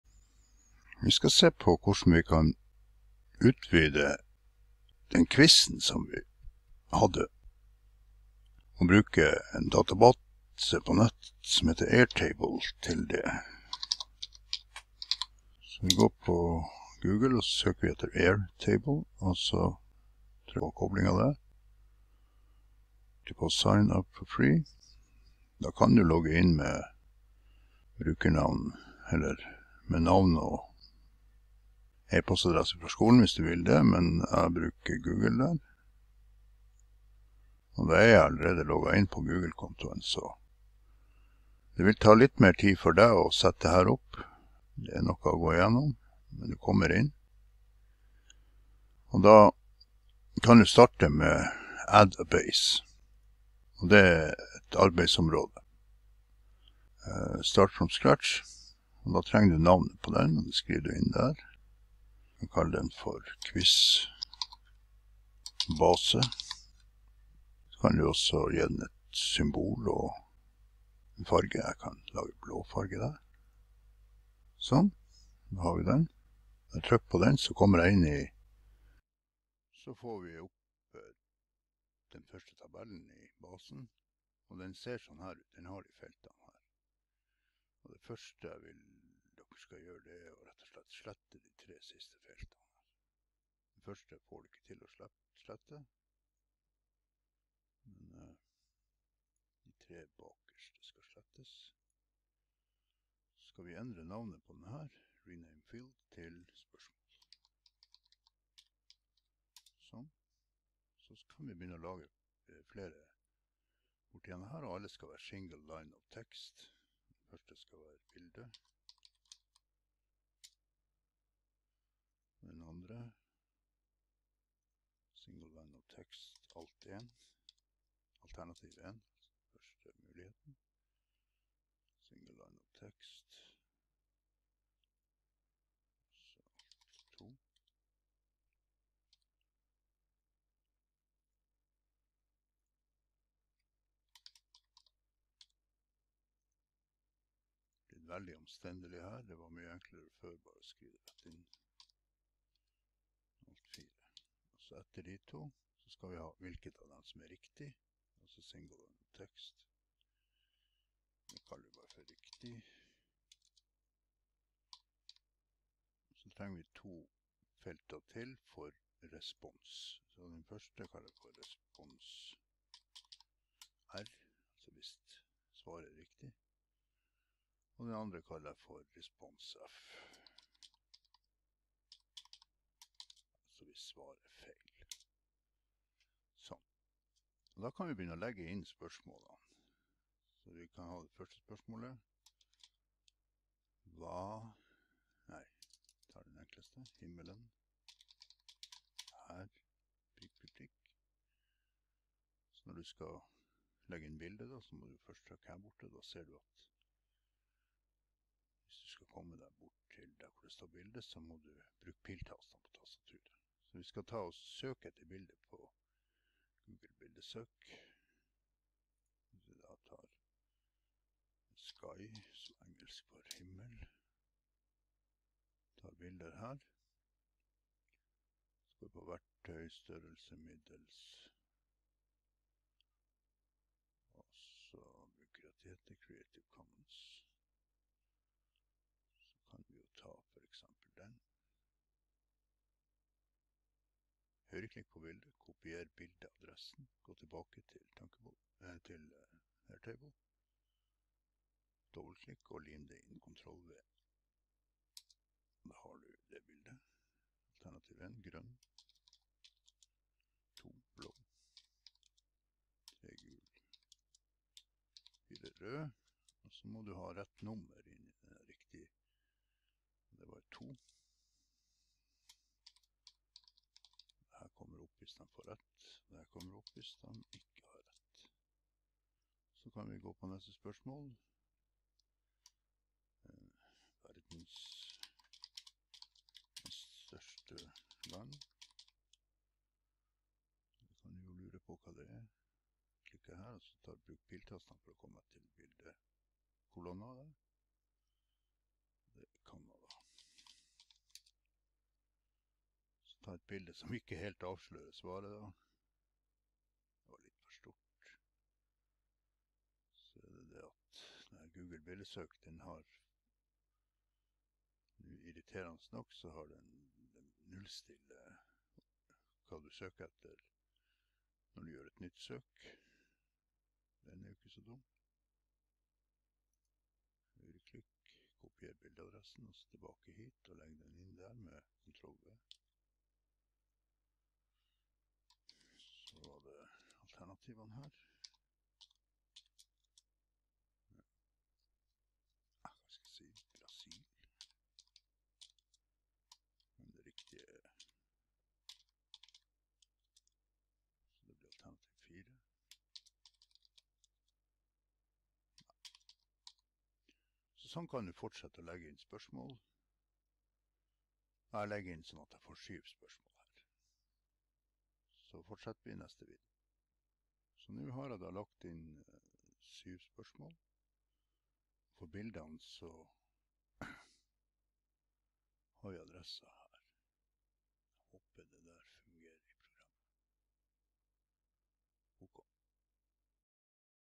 01 Airtable
Starte med AirTable for Thunkable X. Må bare beklage at lyden falt ut i nesten hele videoen, tror likevel det du ser er forståelig. Jeg bruker Google Chrome som nettleser, og kopierer bildeadresse som jeg limer inn i bilde-kolonnene på hvert spørsmål. Etter å ha laget 2 spørsmål ganske sakte, viser jeg den ferdige tabellen med 7 spørsmål.